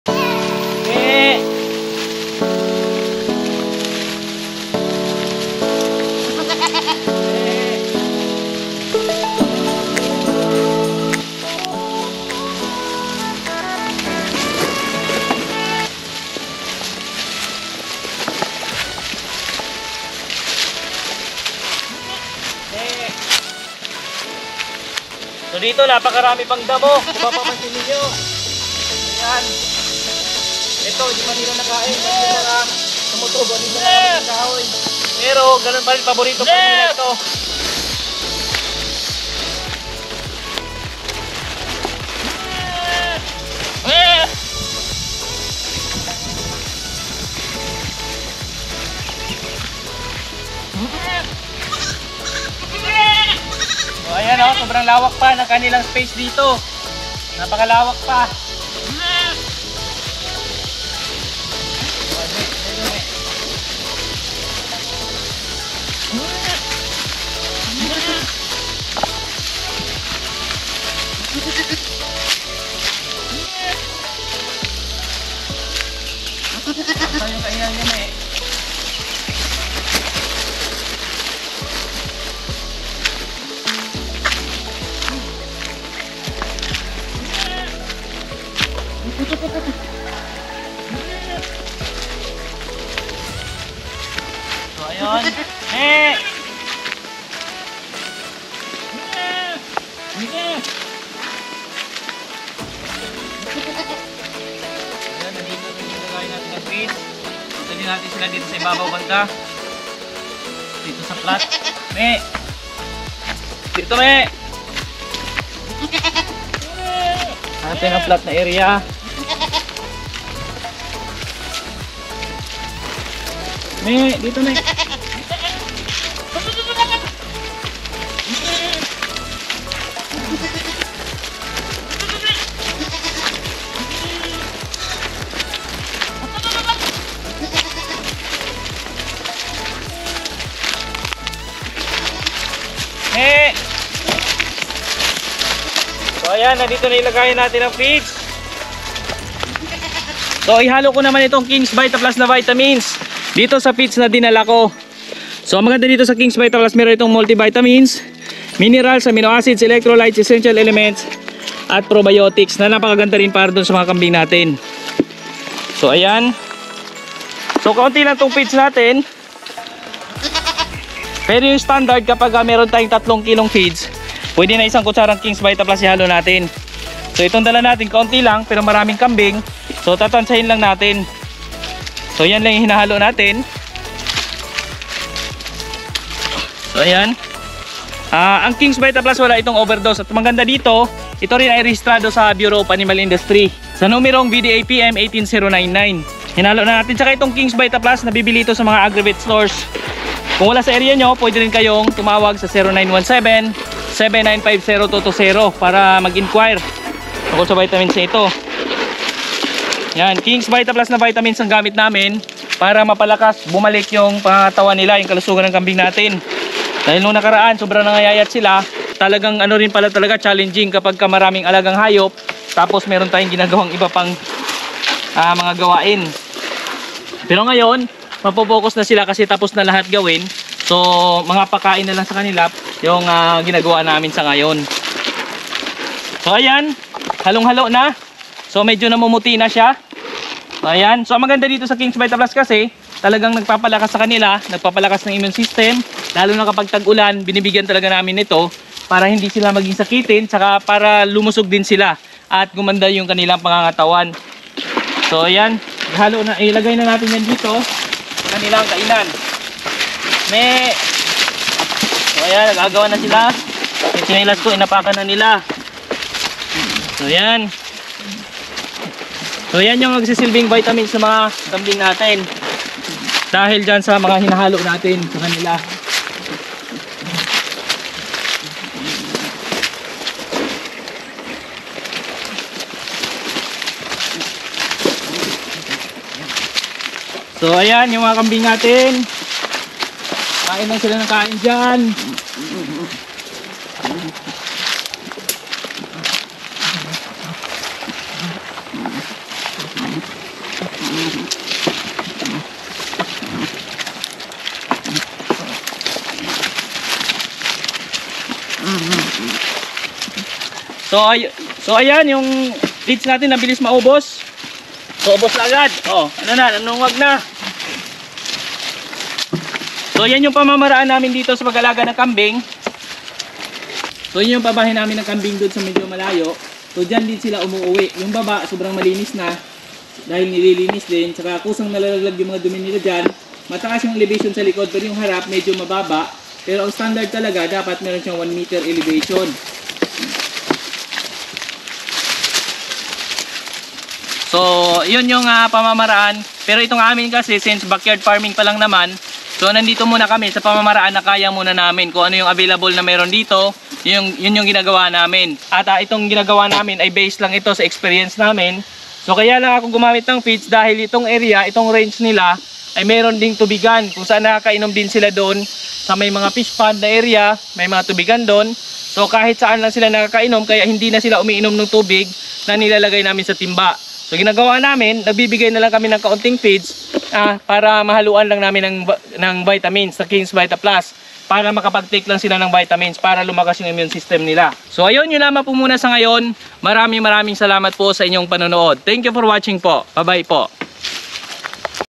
Kambingan Namin Masunurin Lakas Kumain ng Feeds
Kambing